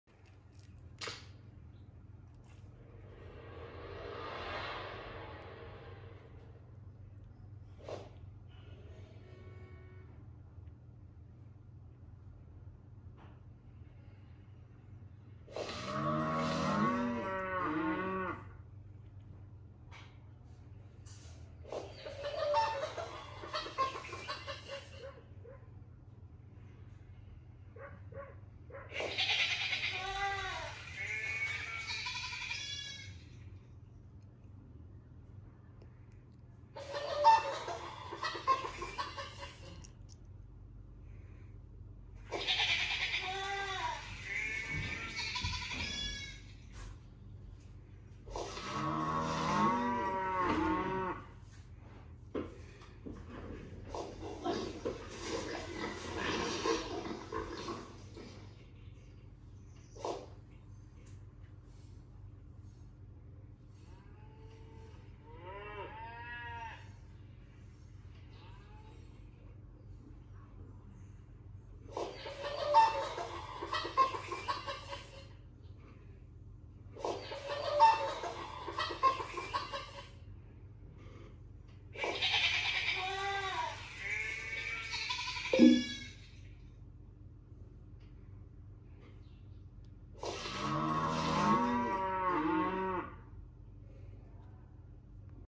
Peeku reacting to other animal sounds
Peeku reacting to other animal sounds. Starts searching for them.